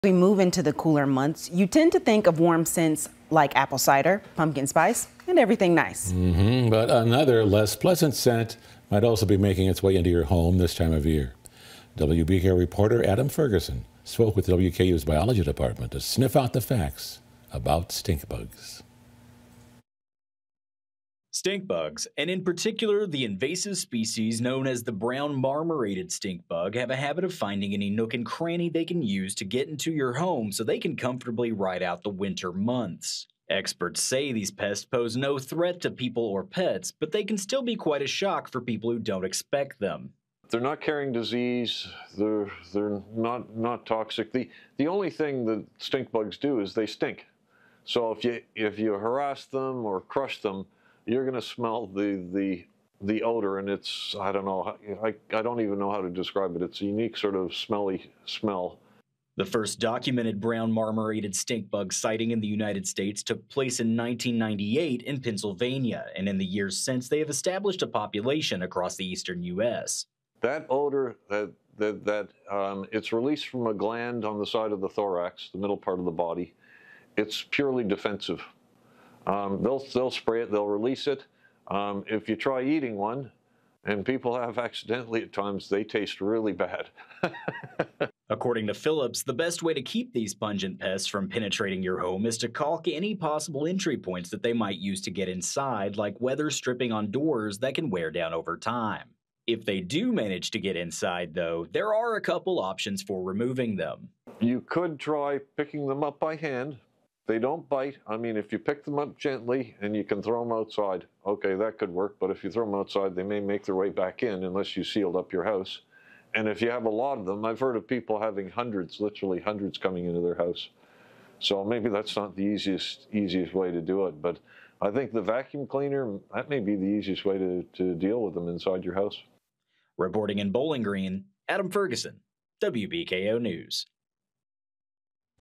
Tips for keeping stink bugs out of your house
Experts say these pests pose no threat to people or pets, but they can still be quite a shock for people who don’t expect them.